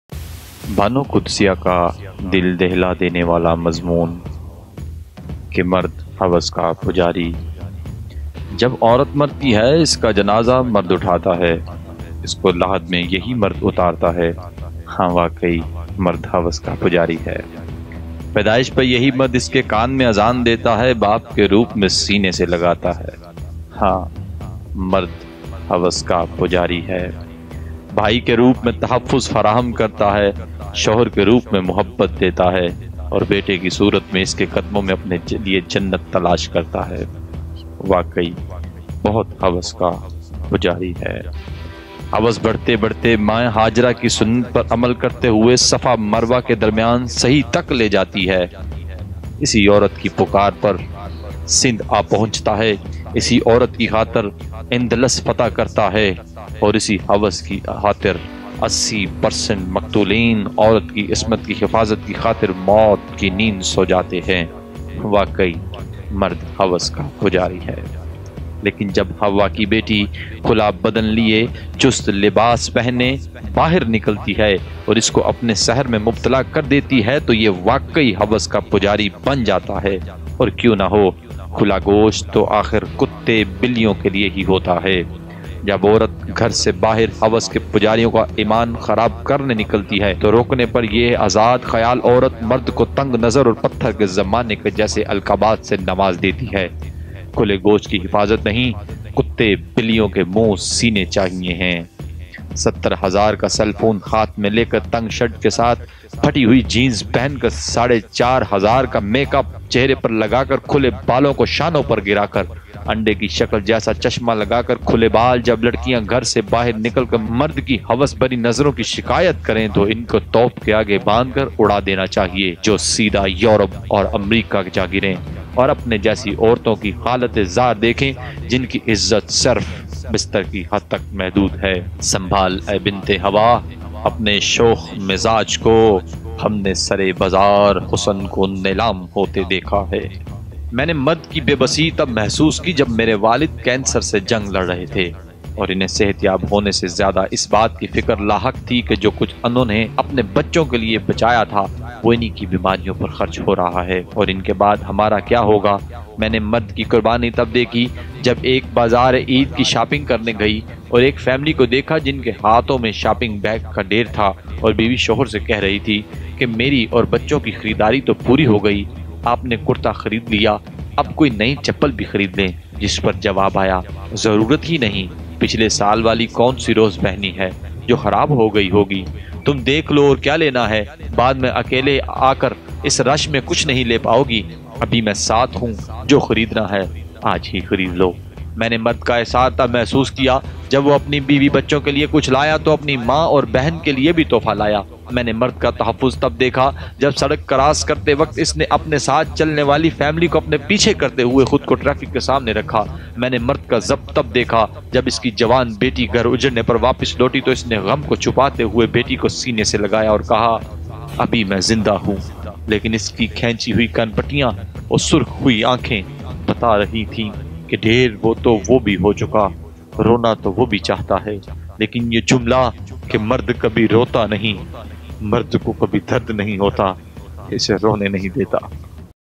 BANO QUDSIA : VERY HEART TOUCHING STORY
CHANNEL MAST QALANDAR
بانو قدسیہ کا دل ہلا دینے والا مضمون

          مردھوسکا_پجاری

جب عورت مرتی ھے اس کا جنازہ مرد اٹھاتا ھے ۔اس کو لحد میں یہی مرد اتارتا ھے ۔۔۔ پیدائش پر یہی مرد اس کے کان میں اذان دیتا ھے۔ باپ کے روپ میں سینے سے لگاتا ھے بھائی کے روپ میں تحفظ فراہم کرتا ھے اور شوہر کے روپ میں محبت دیتا ھے۔ اور بیٹے کی صورت میں اس کے قدموں میں اپنے لیے جنت تلاش کرتا ھے ۔۔۔ واقعی بہت ھوس کی نگاہ سے دیکھتا ھے ۔۔۔ ھوس بڑھتے بڑھتے ماں حاجرہ کی سنت پر عمل کرتے ہوئے صفا و مروہ کے درمیان سعی تک لے جاتی ھے ۔۔۔ اسی عورت کی پکار پر سندھ آپہنچتا ھے ۔۔۔ اسی عورت کی خاطر اندلس فتح کرتا ھے۔ اور اسی ھوس کی خاطر 80% مقتولین عورت کی عصمت کی حفاظت کی خاطر موت کی نیند سو جاتے ہیں۔ واقعی ''مرد ھوس کا پجاری ھے۔''

لیکن جب ھوا کی بیٹی کھلا بدن لیے، چست لباس پہنے باہر نکلتی ھے اور اسکو اپنے سحر میں مبتلا کر دیتی ھے تو یہ واقعی ھوس کا پجاری بن جاتا ھے ۔۔۔ اور کیوں نا ھو؟؟

 کھلا گوشت تو آخر کتے بلیوں کے لیے ھی ھوتا ھے ۔۔۔

 جب عورت گھر سے باھر ھوس کے پجاریوں کا ایمان خراب کرنے نکلتی ھے۔ تو روکنے پر یہ آزاد خیال عورت مرد کو "تنگ نظر" اور "پتھر کے زمانہ کا" جیسے القابات سے نواز دیتی ھے کہ کھلے گوشت کی حفاظت نہیں کتوں بلوں کے منہ سینے چاہیے ھیں

ستر ہزار کا سیل فون ہاتھ میں لیکر تنگ شرٹ کے ساتھ پھٹی ھوئی جینز پہن کر ساڑھے چارہزار کا میک اپ چہرے پر لگا کر کھلے بالوں کو شانوں پر گرا کر انڈے کی شکل جیسا چشمہ لگا کر کھلے بال جب لڑکیاں گھر سے باہر نکل کر مرد کی ھوس بھری نظروں کی شکایت کریں تو انکو توپ کے آگے باندھ کر اڑادینا چاہئیے جو سیدھا یورپ و امریکہ میں جاگریں اور اپنے جیسی عورتوں کی حالت_زار دیکھیں جنکی عزت صرف بستر کی حد تک محدود ھے

"سنبھال اے بنت حوا اپنے شوخ مزاج کو

ھم نے سر_بازار حسن کو نیلام ھوتے دیکھا ھے"
2

مرد

میں نے مرد کی بے بسی تب محسوس کی جب میرے والد کینسر سے جنگ لڑ رہے تھے اور انھیں صحت یاب ہونے سے زیادہ اس بات کی فکر لاحق تھی کہ جو کچھ انھوں نے اپنے بچوں کے لئے بچایا تھا وہ ان کی بیماری پر خرچ ہورہا ھے اور ان کے بعد ھمارا کیا ھوگا؟ میں نے مرد کی قربانی تب دیکھی جب ایک بازارعید کی شاپنگ کرنے گئی اور ایک فیملی کو دیکھا جن کے ھاتھوں میں شاپنگ بیگز کا ڈھیر تھا اور بیوی شوہر سے کہہ رھی تھی کہ میری اور بچوں کی خریداری پوری ھوگئی آپ نے کرتا خرید لیا اپ کوئی نئی چپل بھی خرید لیں جس پر جواب آیا ضرورت ہی نہیں پچھلے سال والی کونسی روز پہنی ھے جو خراب ھوگئی ھوگی، تم دیکھ لو اور کیا لینا ھے بعد میں اکیلے آکر اس رش میں کچھ نہیں لے پاو گی۔ ابھی میں ساتھ ھوں جو خریدنا ھے آج ھی خرید لو۔
میں نے مرد کا ایثار تب محسوس کیا جب وہ اپنی بیوی بچوں کے لئے کچھ لایا تو اپنی ماں اور بہن کے لئے بھی تحفہ لایا، میں نے مرد کا تحفظ تب دیکھا جب سڑک کراس کرتے وقت اس نے اپنے ساتھ چلنے والی فیملی کو اپنے پیچھے کرتے ہوئے خود کو ٹریفک کے سامنے رکھا۔ میں نے مرد کا ضبط تب دیکھا جب اس کی جوان بیٹی گھر اجڑنے پر واپس لوٹی تو اس نے غم کو چھپاتے ھوئے بیٹی کو سینے سے لگایا اور کہا کہ ابھی میں زندہ ھوں لیکن اس کی کھنچتی ہوئے کنپٹیاں اور سرخ ھوتی ھوئی آنکھیں بتارھی تھیں کہ ڈھیر تو وہ بھی ھوچکا، رونا تو وہ بھی چاہتا ہے لیکن یہ جملہ کہ مرد کبھی روتا نہیں ھے اسے رونے نہیں دیگا۔

۔ ( بانو قدسیہ)